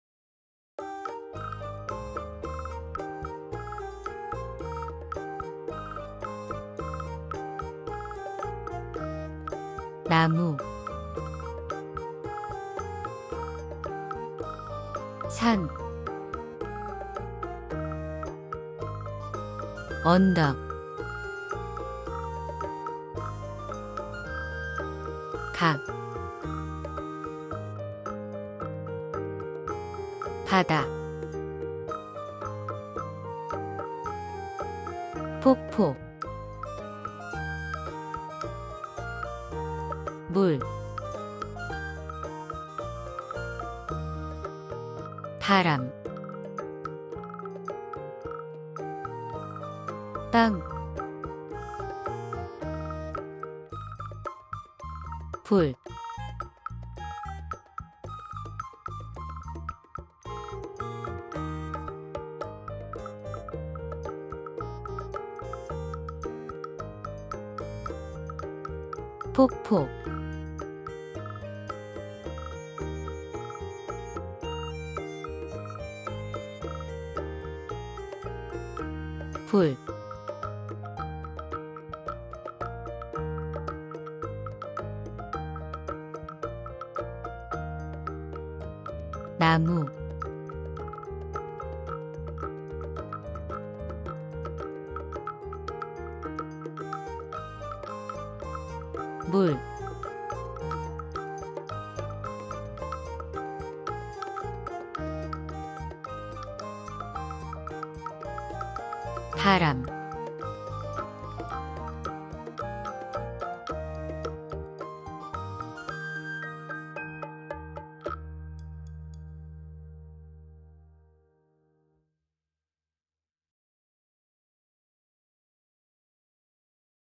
Korean Vocabulary Practice for beginner learners [Nature]ㅣ한국어 기초 어휘
Do you like nature?
Watch this video and learn various Korean vocabulary!